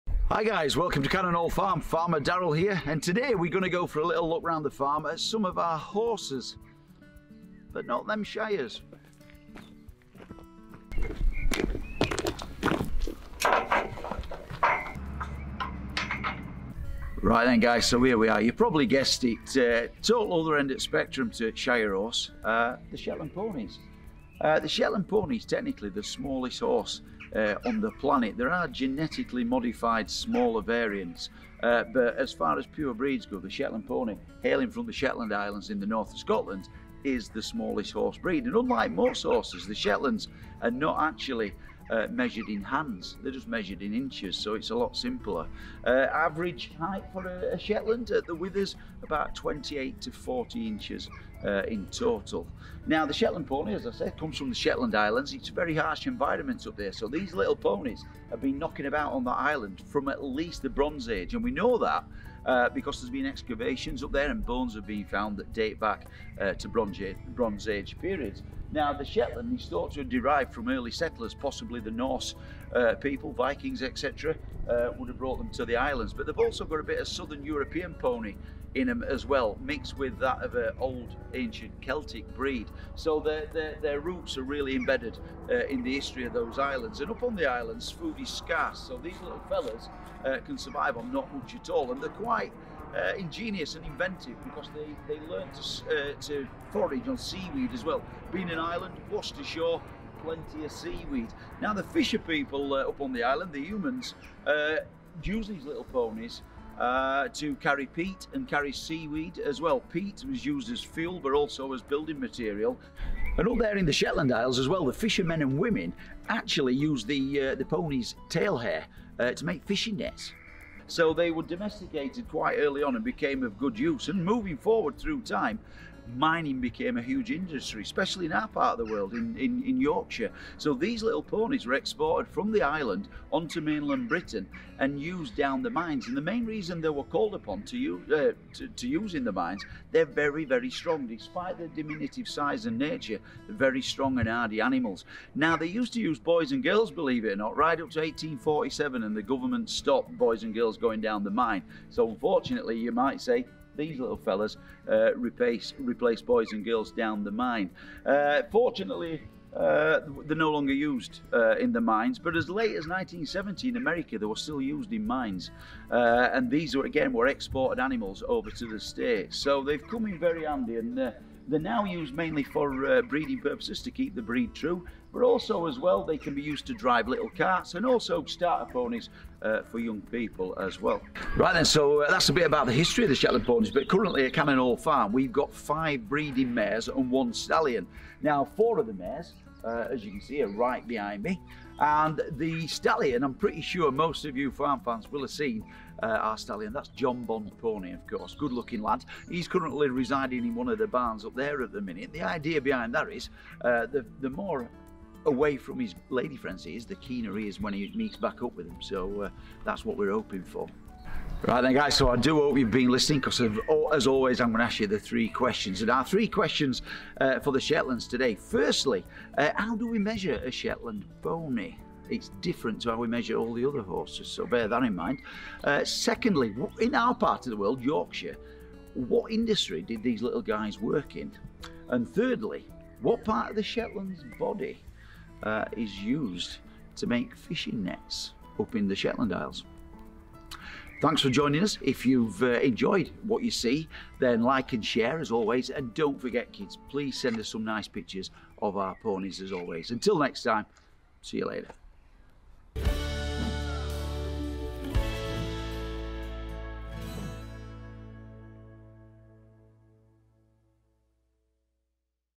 ** JUST FOR KIDS - OUR SHETLAND PONIES **
Join Farmer Darrell in our latest installment of educational videos from teamchf as he tells you all about our Shetland Ponies that appeared on This Week On the Farm.
Join Cannon Hall Farm online now for plenty of other animal news and videos.